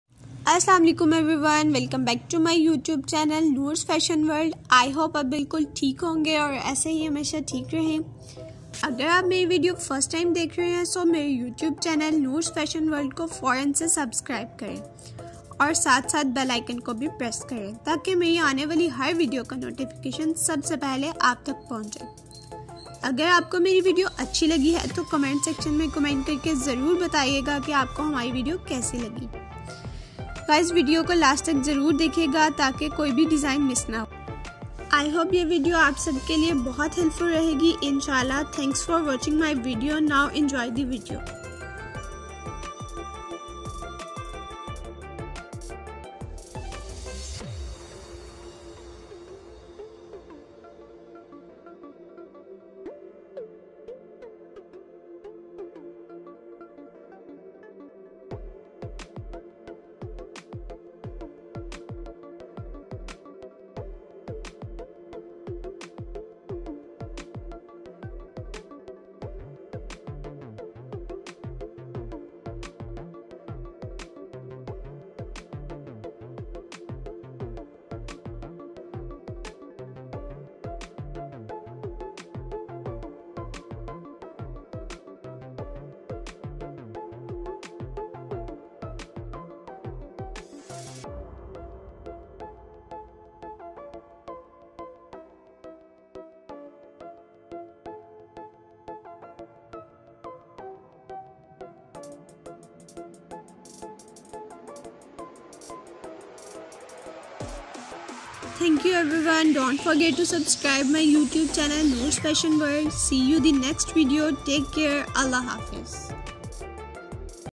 40+ Trendy sleeves designs for shirt...Elegant Stitching pattern....Designer Winter collection 2022.
Hey u r watching Trendy sleeves designs for shirt...Elegant Stitching pattern. Designer Winter collection 2022

Designing on sleeves with tissue tassles pearls n many many more. Stunning collection.